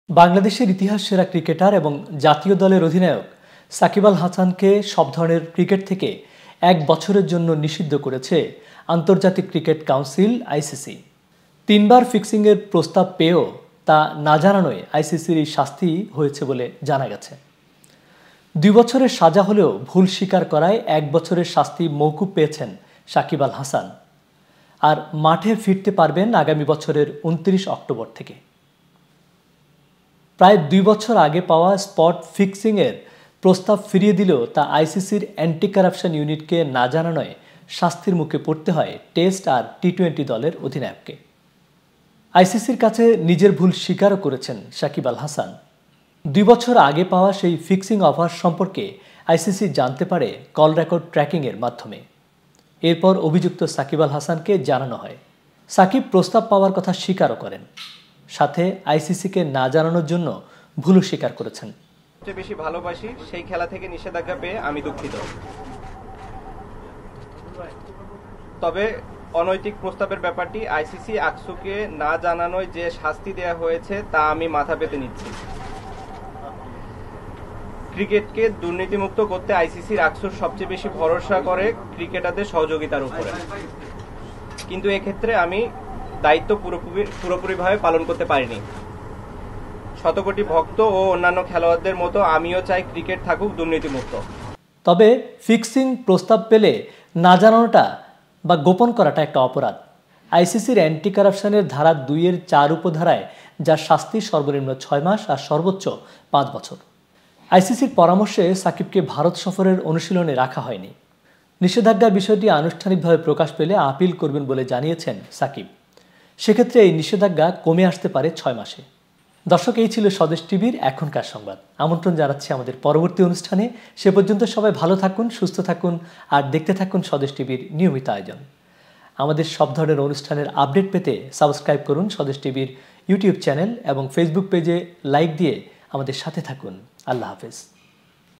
সাকিবকে ১ বছর নিষিদ্ধ করেছে আইসিসি
বাংলাদেশের ইতিহাস সেরা ক্রিকেটার এবং জাতীয় দলের অধিনায়ক সাকিব আল হাসানকে সব ধরণের ক্রিকেট থেকে এক বছরের জন্য নিষিদ্ধ করেছে আন্তর্জাতিক ক্রিকেট কাউন্সিল (আইসিসি)। তিনবার ফিক্সিংয়ের প্রস্তাব পেয়েও না জানানোয় আইসিসির এই শাস্তি দিয়েছে বলে জানা গেছে। দুই বছরের সাজা হলেও ভুল স্বীকার করায় এক বছরের শাস্তি মওকুফ পেয়েছেন সাকিব আল হাসান। মাঠে ফিরতে পারবেন আগামী বছরের ২৯ অক্টোবর থেকে।